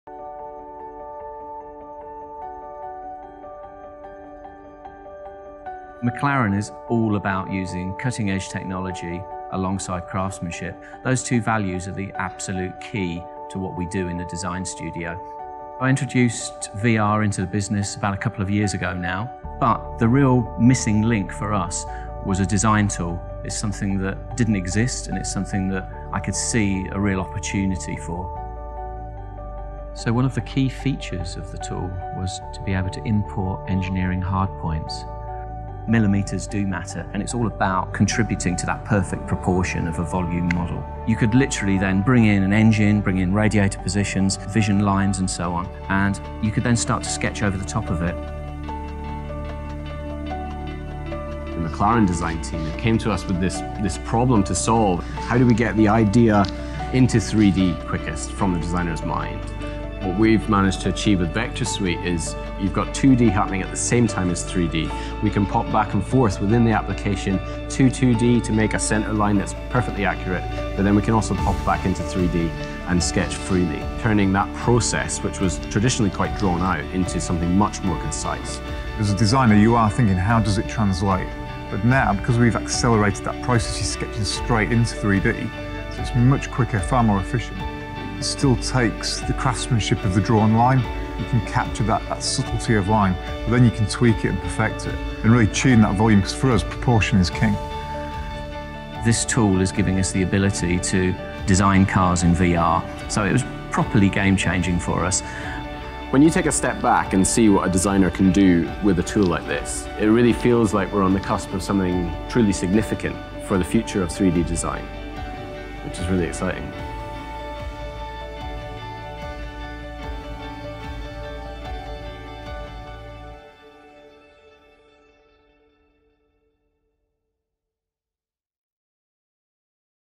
How McLaren Automotive uses virtual reality to design its sportscars and supercars
McLaren Automotive works with software start-up, Vector Suite, to fast-track sportscar and supercar design